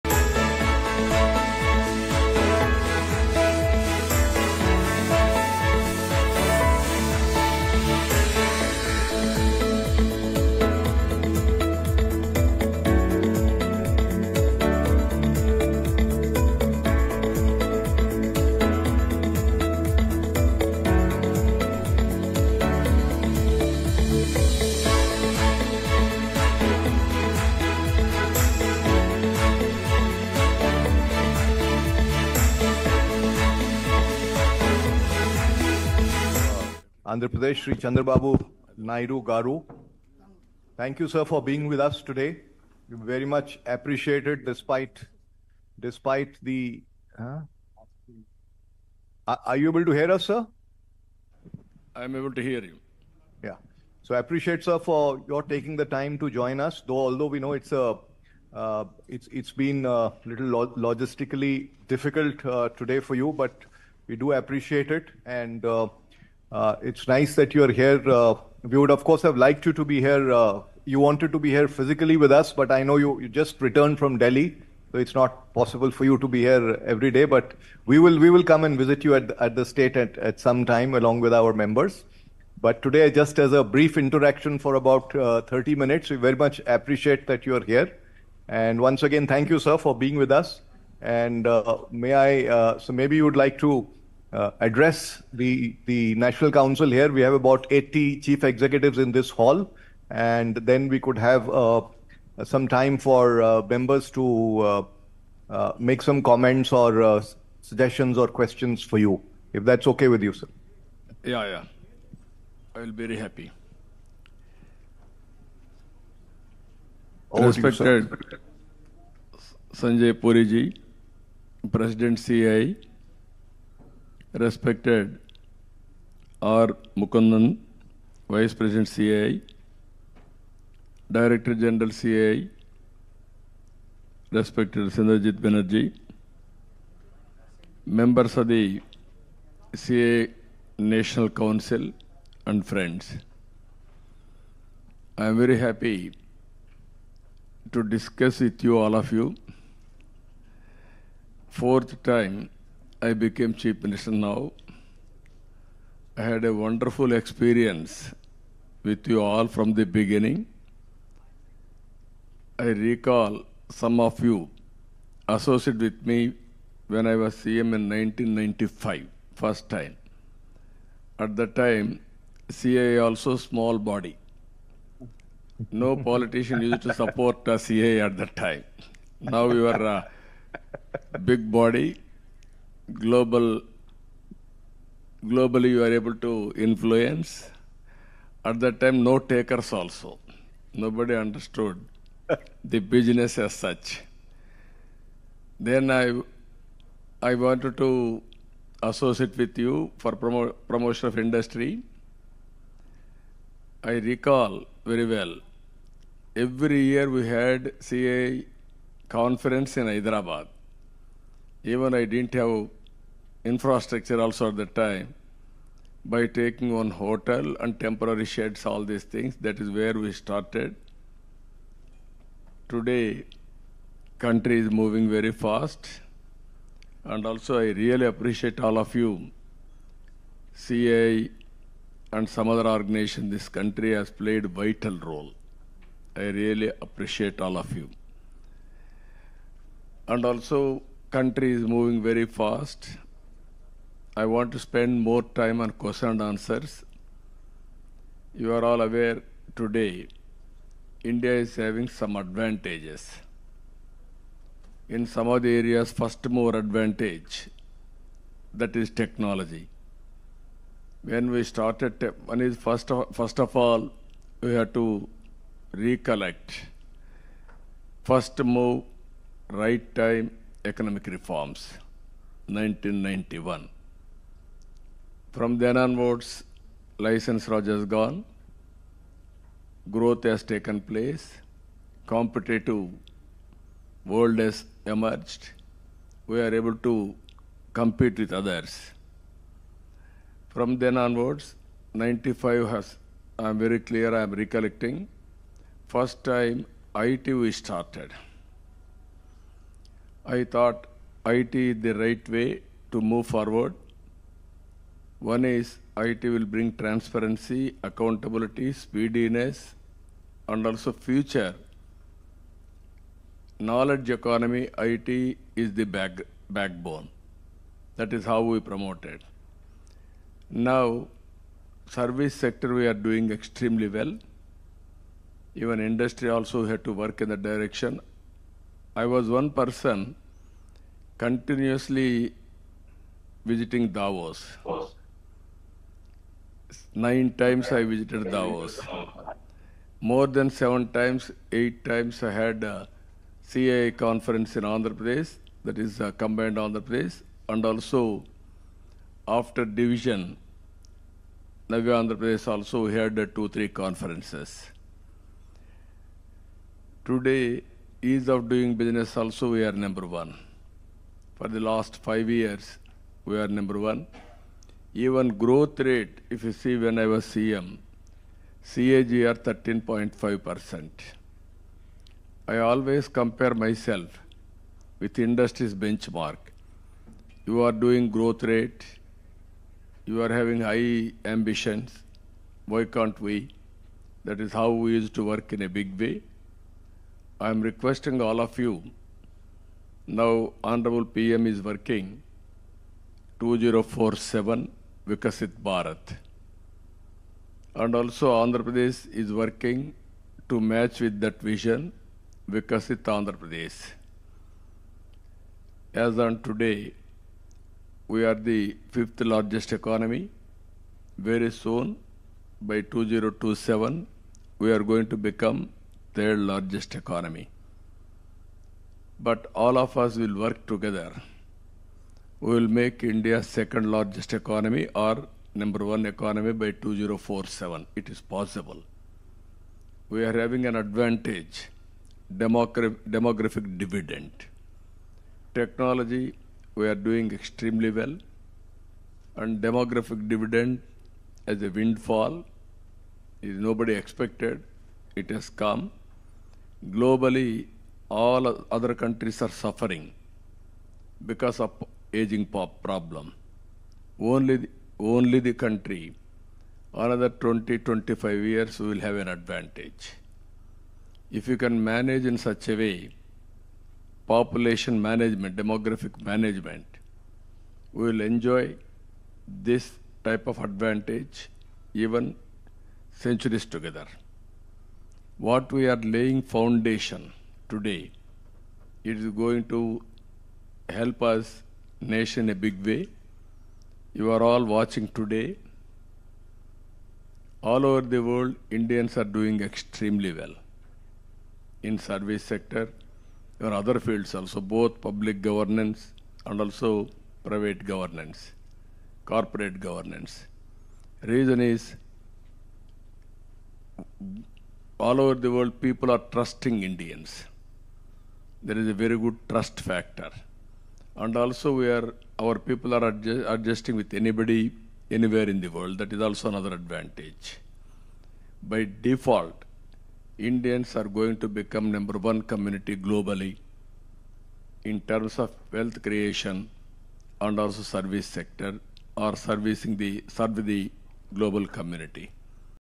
CM Nara Chandrababu Naidu speaking at CII National Council Meeting : Varadhi News
| Varadhi News